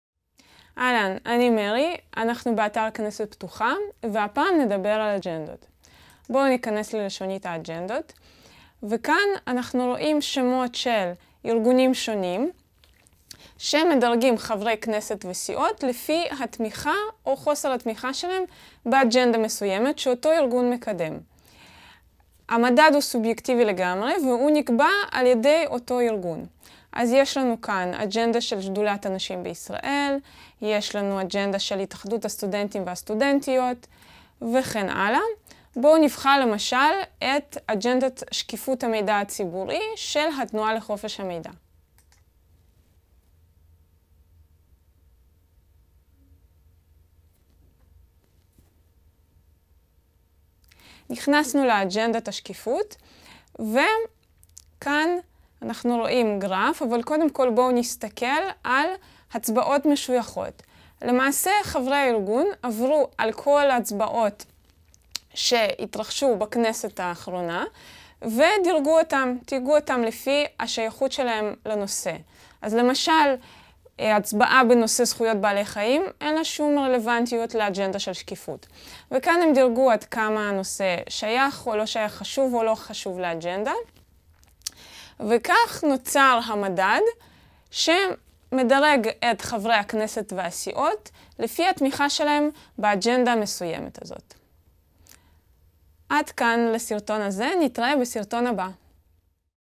הדרכה על "כנסת פתוחה" - 4. אג'נדות
אתר כנסת פתוחה מאפשר לעקוב אחרי המתרחש בכנסת - פעילות הח"כים, תהליך החקיקה ועבודתת הוועדות. זהו פרוייקט התנדבותי של עמותת "הסדנא לידע ציבורי"